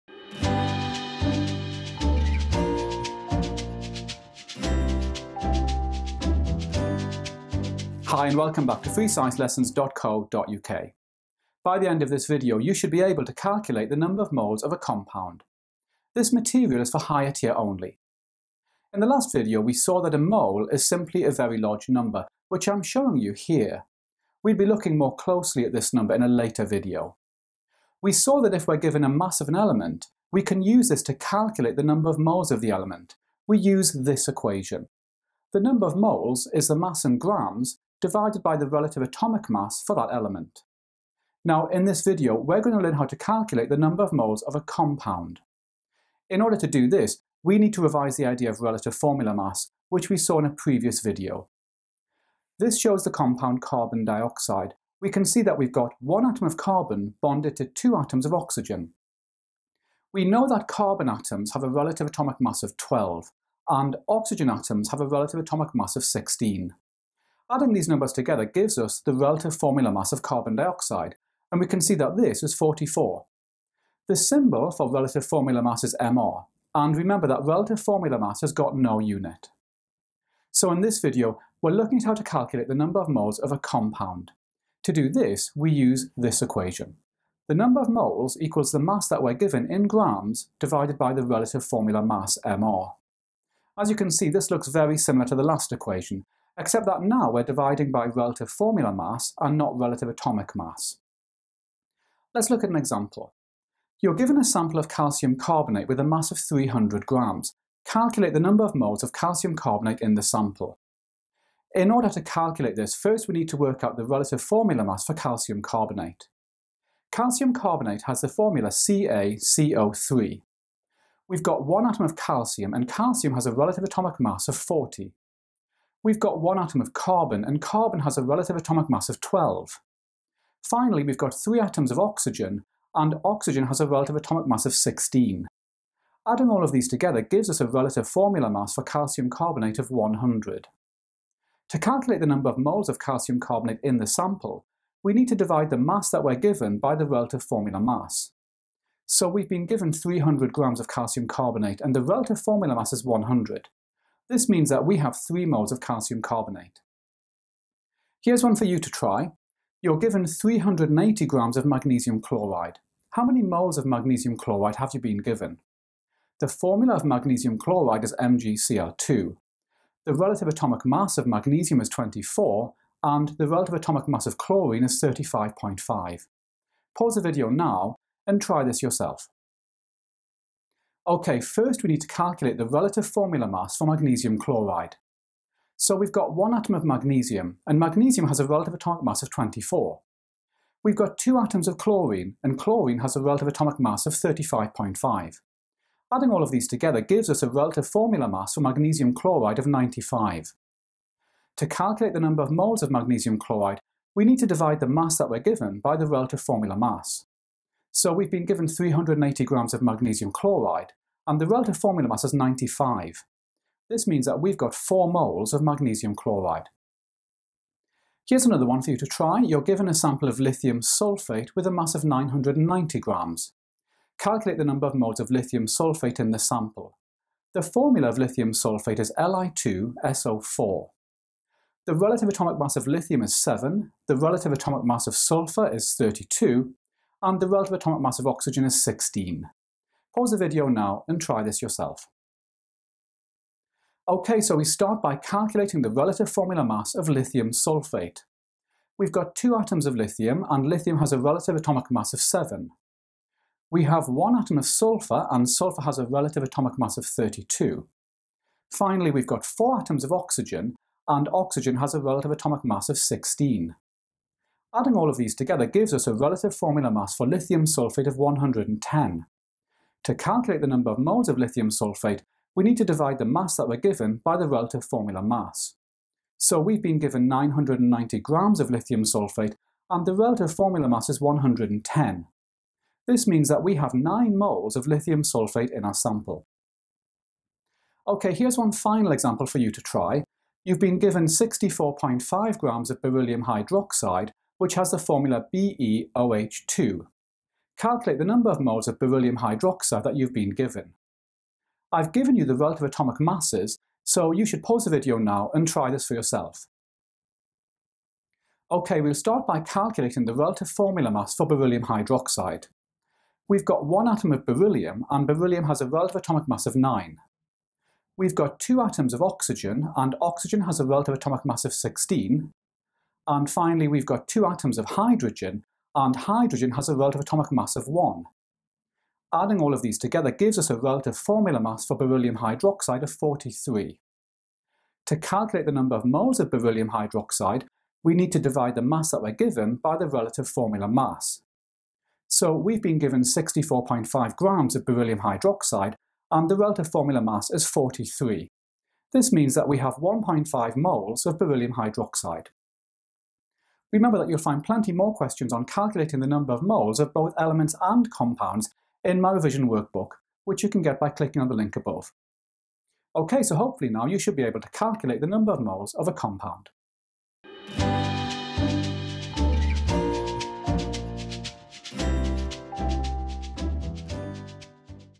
GCSE Chemistry Revision "Calculating Moles of a Compound"
In this video, we continue looking at the idea of moles. We learn how to use the relative formula mass to calculate the number of moles of a compound. You'll need this later, so it's really important material.